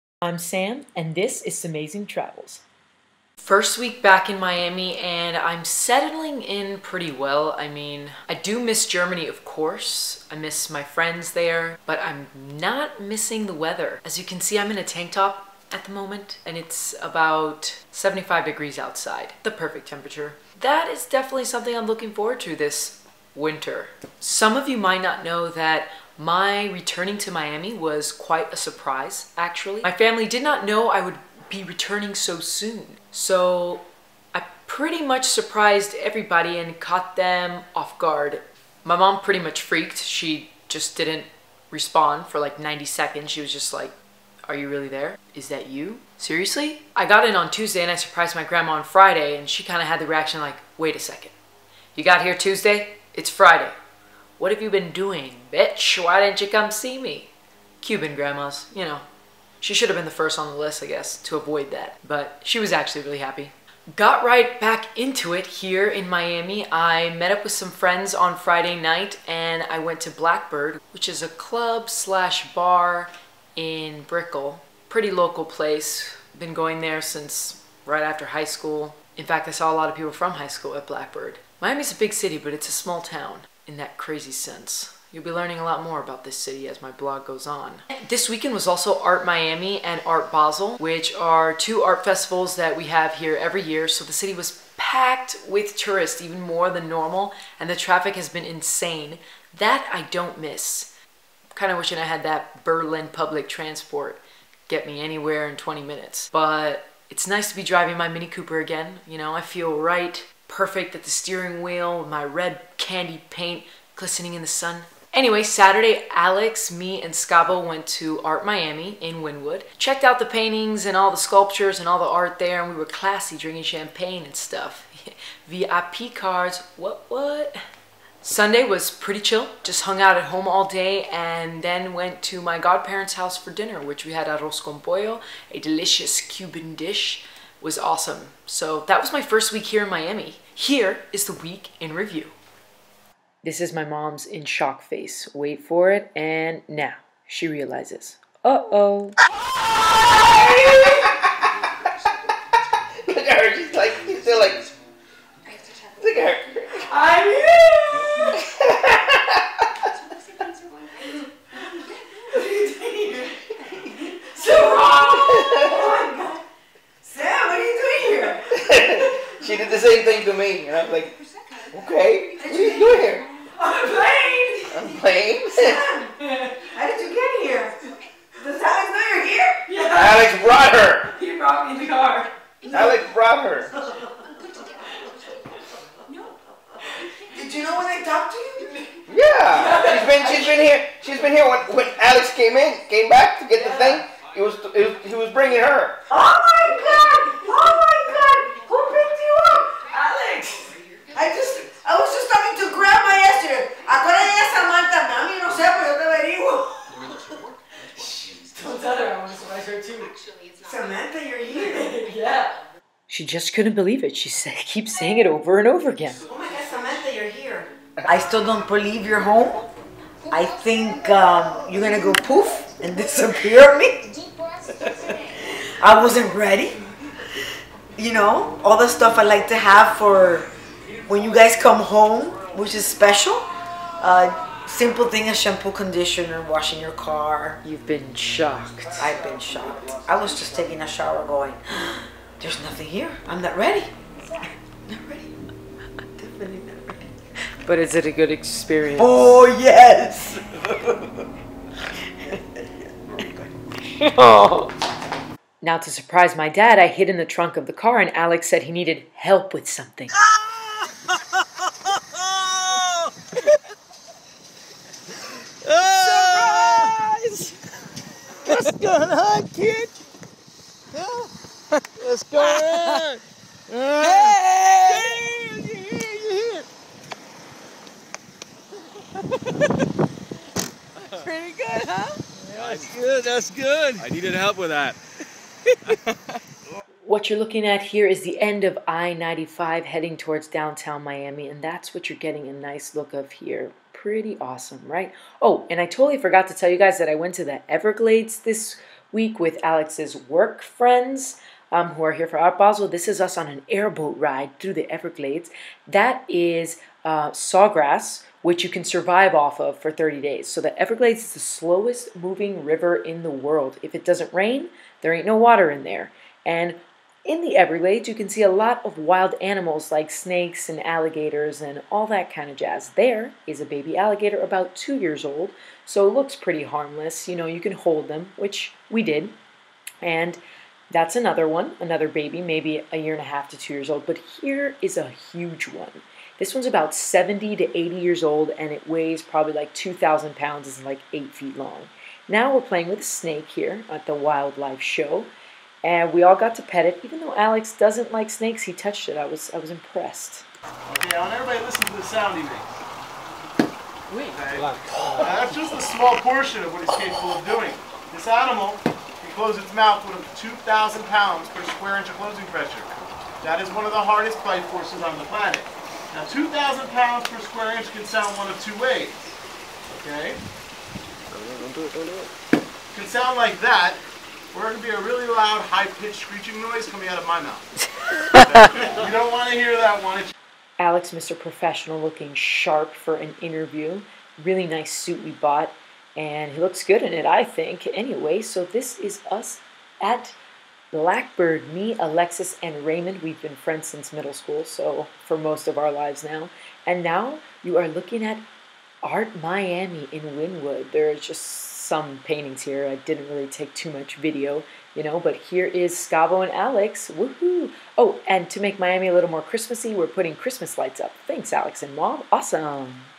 Miami welcomes me home!
After a 2 year hiatus in Berlin, I am back home in the 305. Miami has changed since I have been away and so have I. So that means it is time to explore and discover anew!